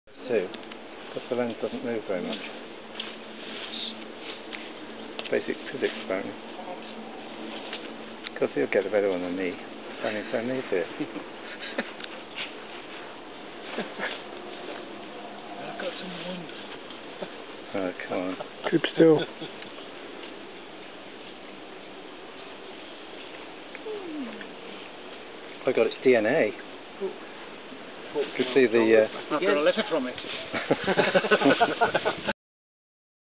Butterflies of the Gambia - Common Dotted Border.avi
wingspan bird tours in the Gambia - Common Dotted Border filmed in Abuko nature Reserve